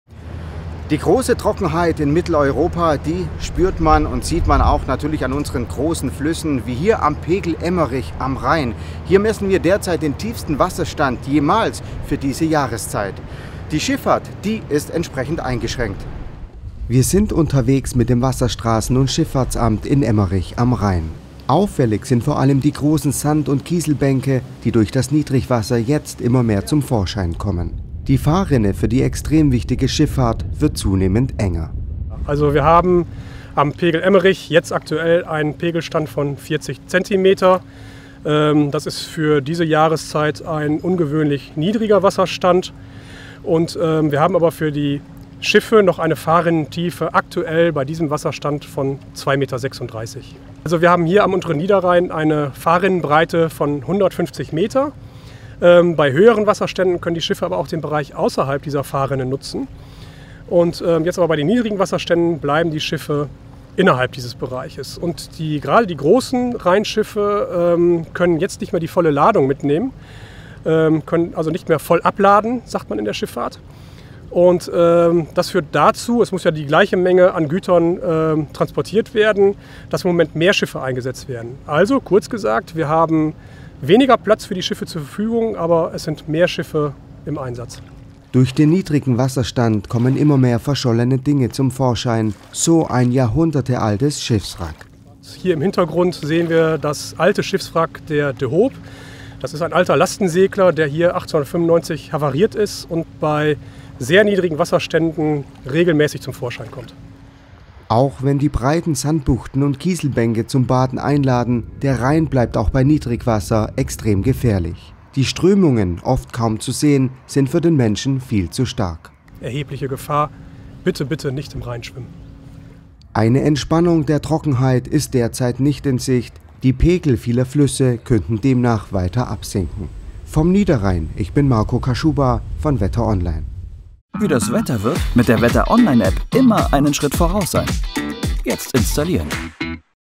Niedrigwasser am Rhein - Schifffahrt bereits eingeschränkt
Am Rhein, wie am Pegel Emmerich, erreichen wir bereits jetzt Rekordniedrigwasser für diese Jahreszeit. Sinkt er weiter, könnte man vielleicht schon bald den Allzeitrekord von 2018 brechen, und zwar viel früher als damals.